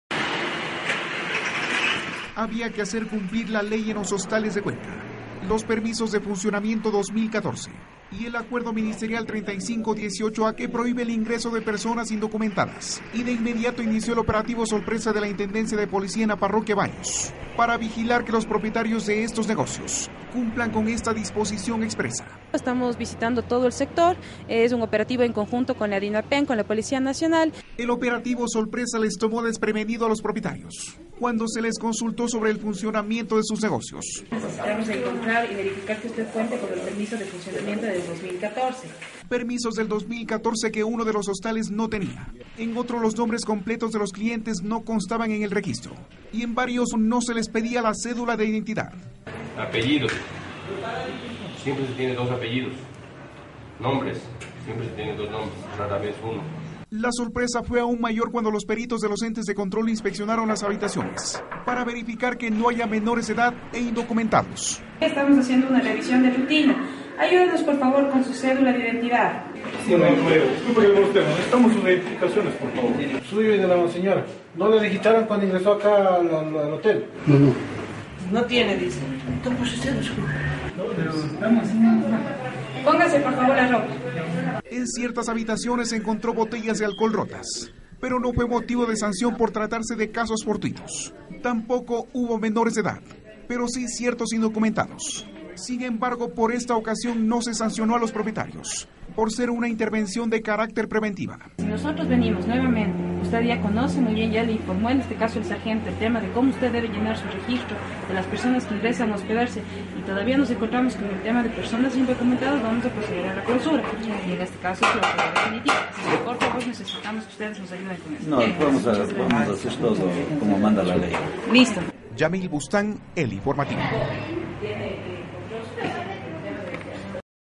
INDOCUMENTADOS Y MENORES DE EDAD NO PUEDEN INGRESAR A HOSTALES
LOS INDOCUMENTADOS Y MENORES DE EDAD NO PUEDEN INGRESAR A LOS HOSTALES TRAS NUEVA DISPOSICIÓN MINISTERIAL, PARA VERIFICAR SI ESTO SE CUMPLE LA INTENDENCIA DE POLICÍA REALIZÓ UN OPERATIVO AL SUR DE CUENCA, LES TENEMOS DETALLES AL RESPECTO….